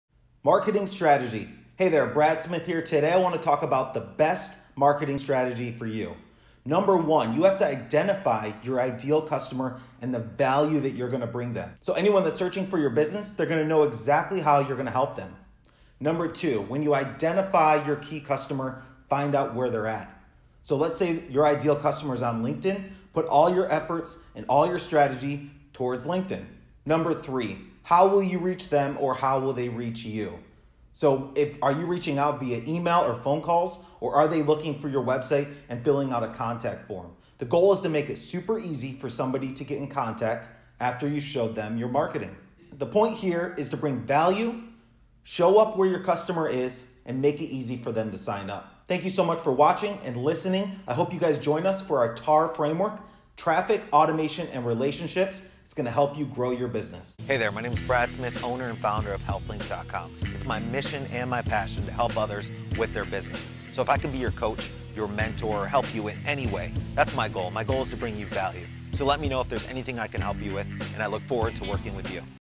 Marketing Strategy By Brad Smith
In this video, audio and blog Brad Smith talks about how to setup your marketing strategy. This will allow new customers to find your website. Once someone finds your website make sure to make it easy to sign-up.

1. What’s the core value you bring to your customers?
2. Who–exactly–is your ideal customer? Can you describe/identify them in great detail?
3. How will you reach them or how will they reach you?